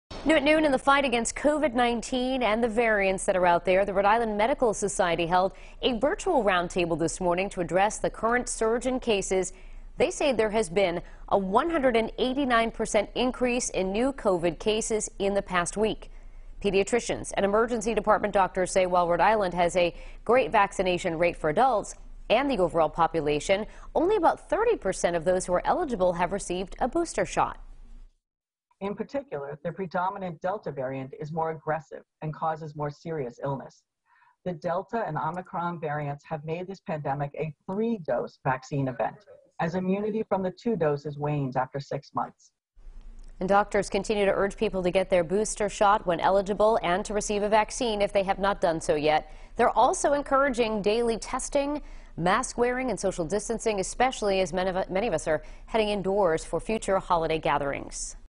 RI Medical Society addresses rise in COVID-19 cases, ways to prevent illness
Members of the RI Medical Society discussed the current surge in COVID-19 cases and how the state and individuals can protect their own health as well as the health of family and friends.